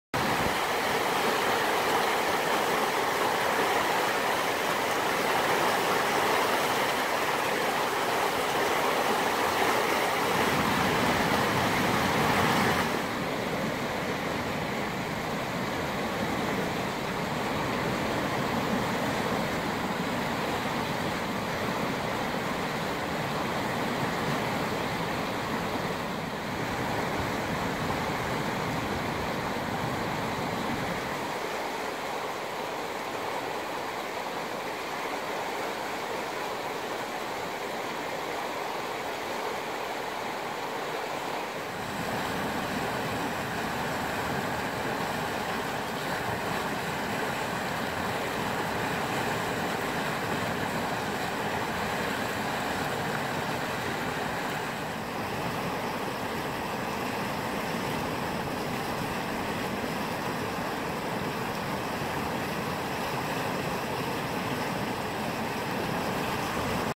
Holzbuhnen
Holzbuhnen sollen den Uferweg entlang der Aare zwischen Parkplatz und Muribad schützen.
Zur Zeit (November 2018) lenkt aber eine Buhne viel Wasser Richtung Ufer.